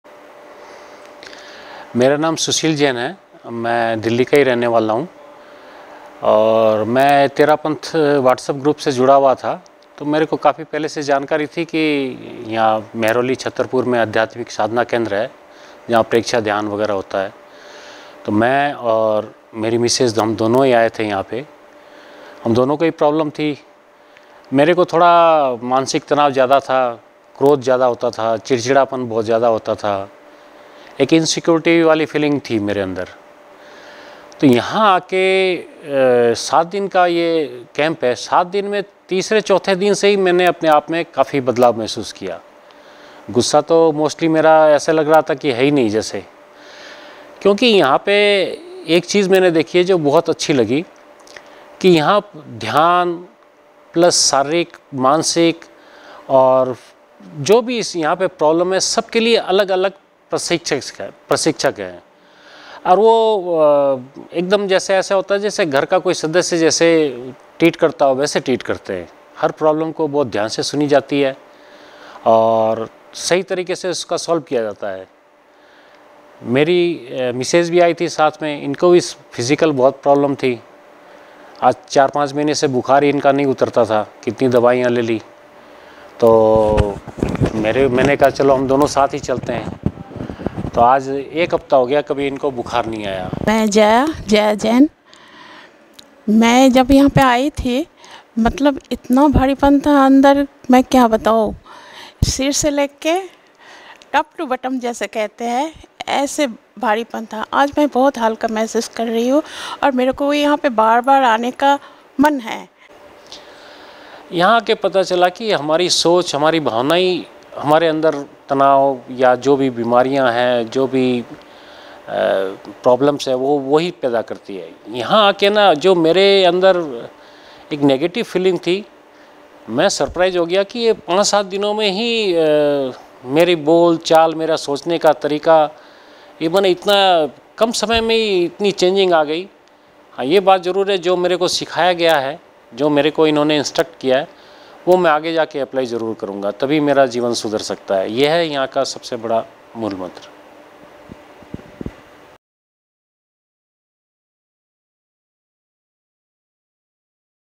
Constant fever evaporates in no time
Preksha Dhyan relief yourself from negative feelings like fear, insecurity etc. and help to enhance positive energy in you to achieve  healthy body and mind.